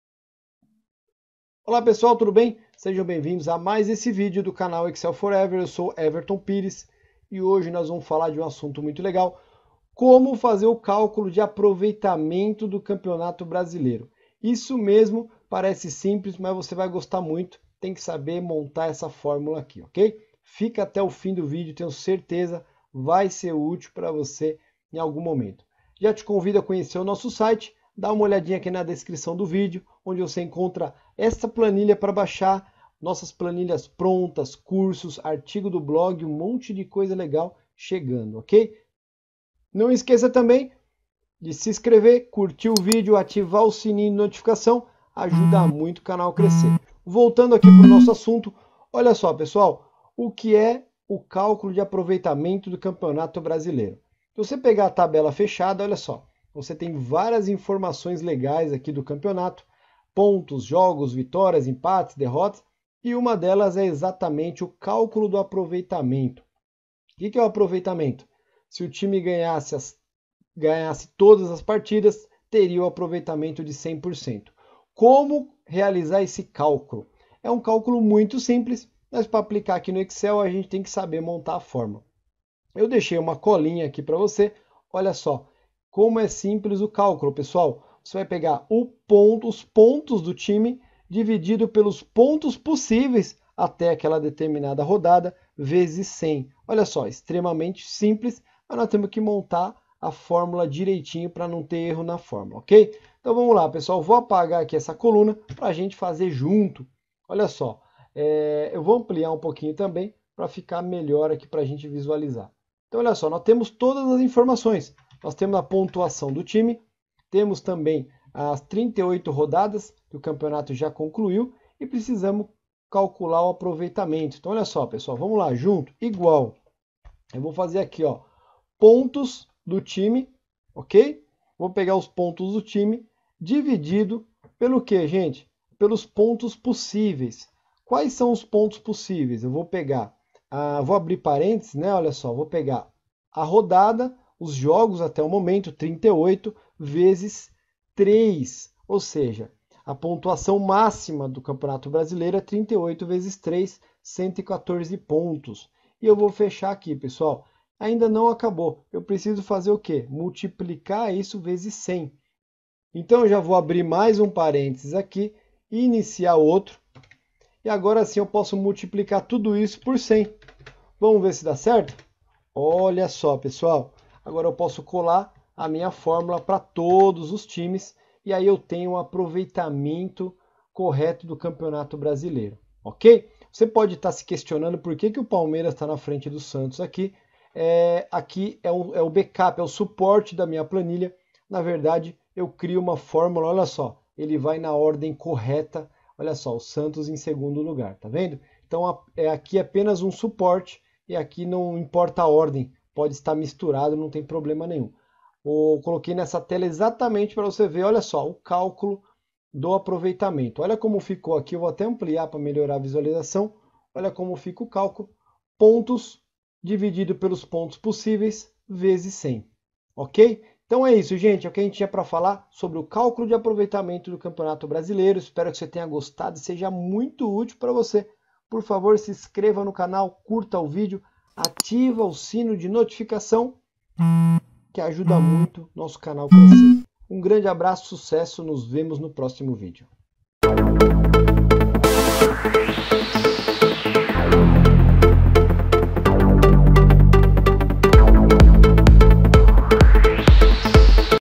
Cálculo de aproveitamento do Campeonato Brasileiro
Veja neste vídeo como calcular o aproveitamento dos times do Campeonato Brasileiro de futebol.